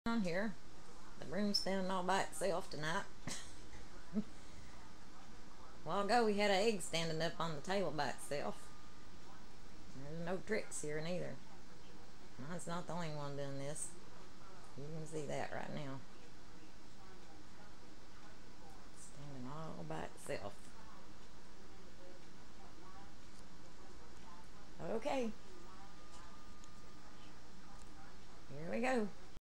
Broom Standing On Its Own (February 29th, 2012)
My broom is standing on its own in my kitchen.  This is without any wires or other tricks.